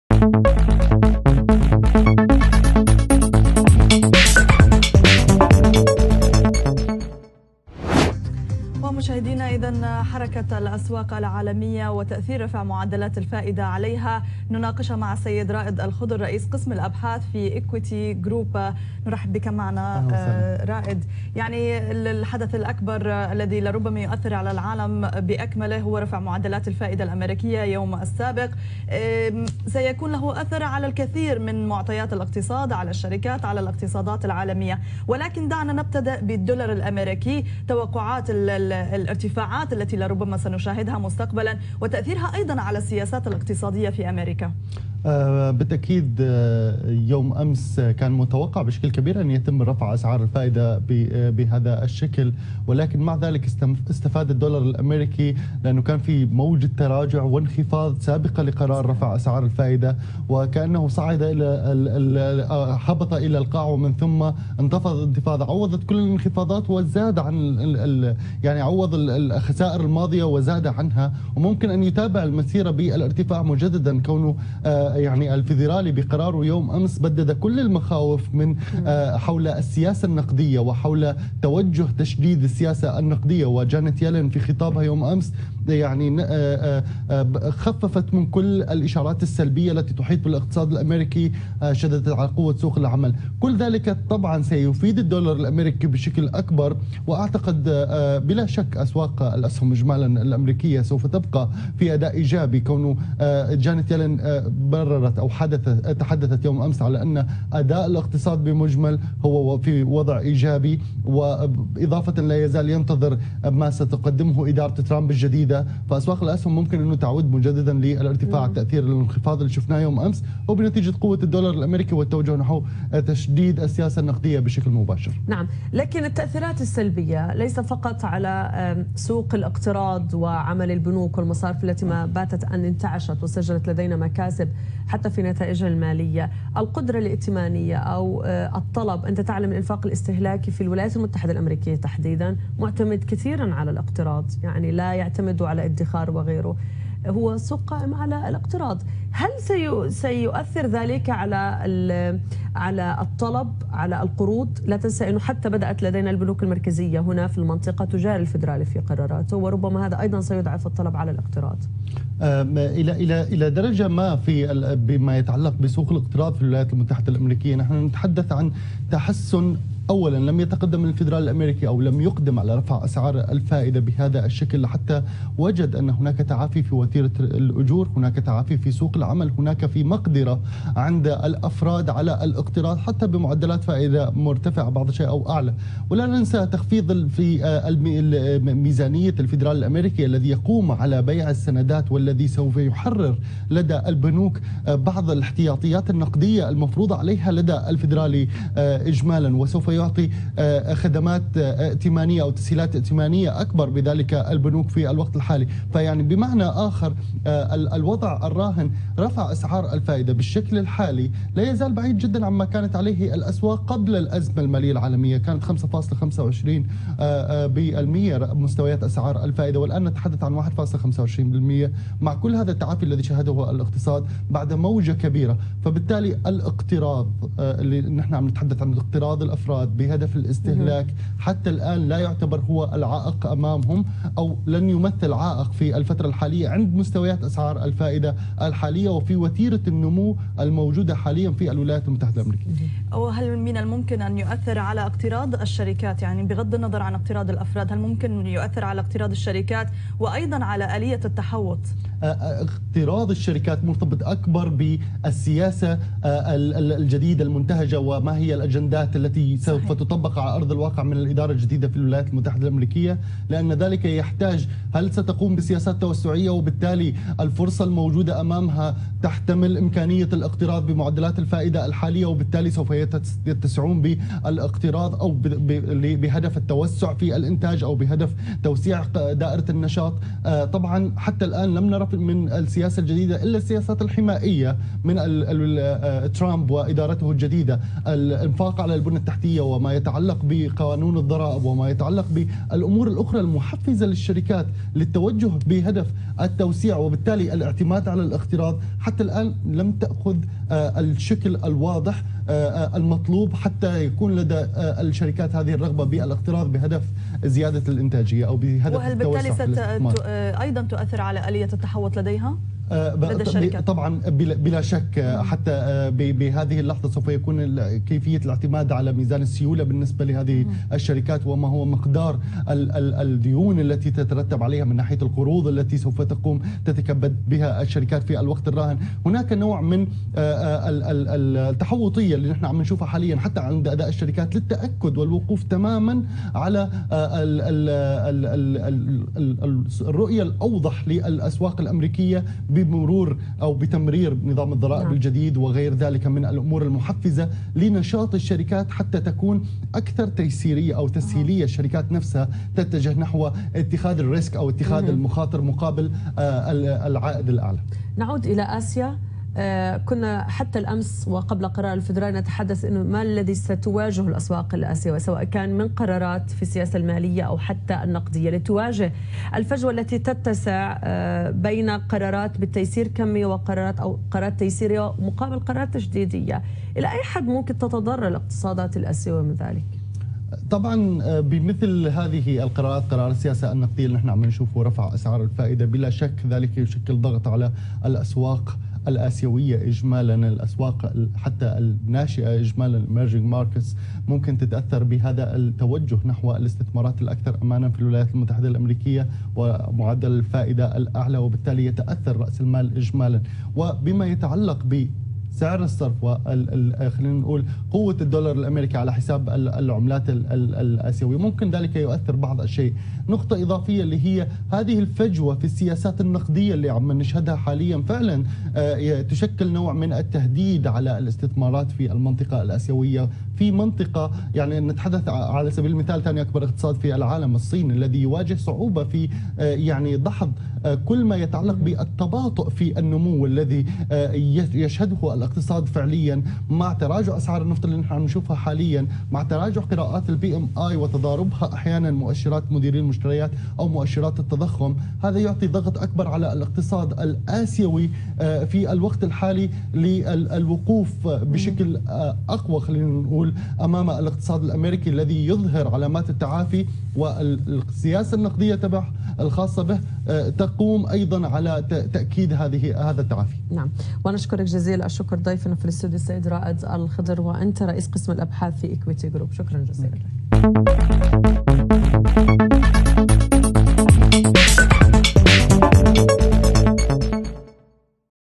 الفيدرالي الأمريكي بخطوات واثقة يرفع سعر الفائدة للمرة الرابعة في عام و نصف
لقاء رئيس قسم الأبحاث في Equiti Group رائد الخضر @raedkhed على  قناة CNBC العربية